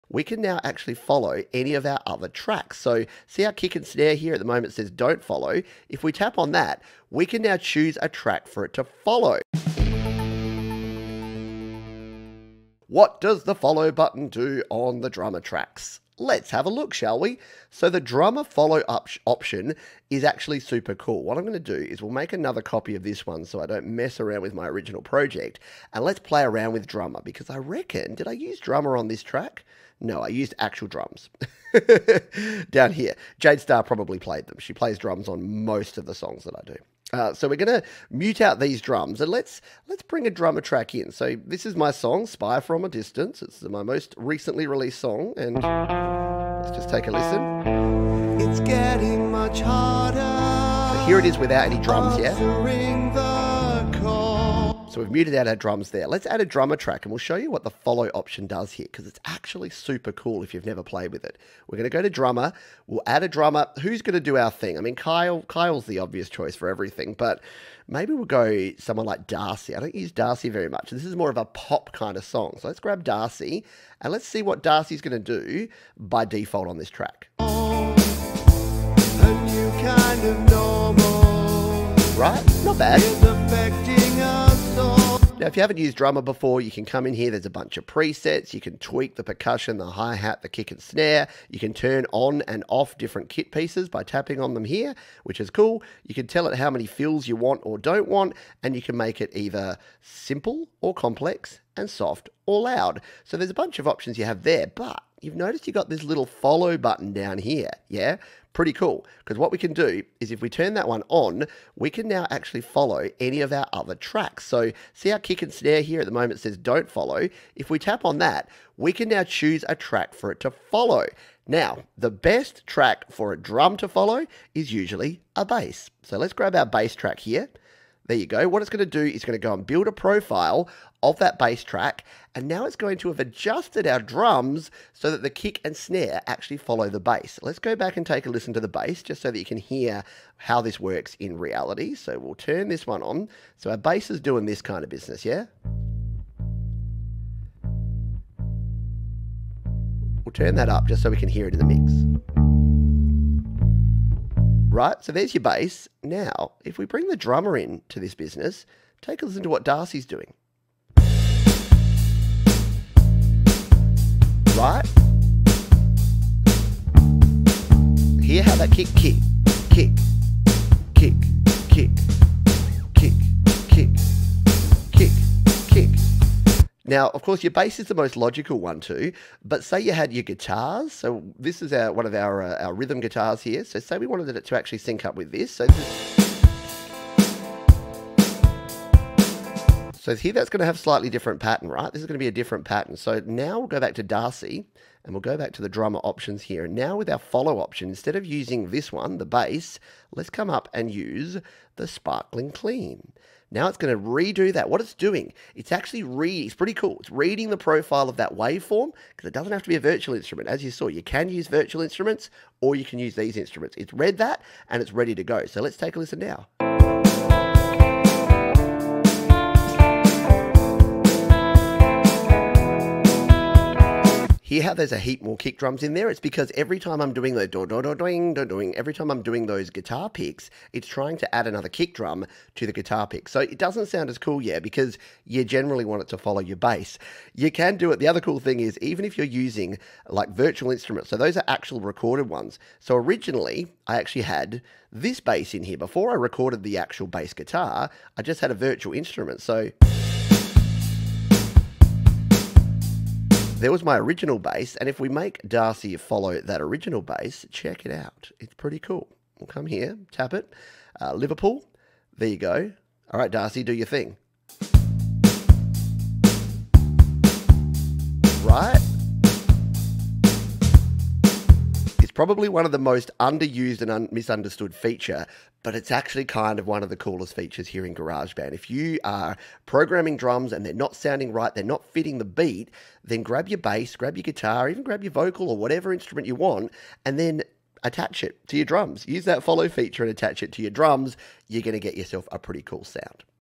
Virtual Drummer FOLLOW option in GarageBand iOS (iPad/iPhone)
How to use the FOLLOW option on the virtual drummer in GarageBand iPad and GarageBand iPhone.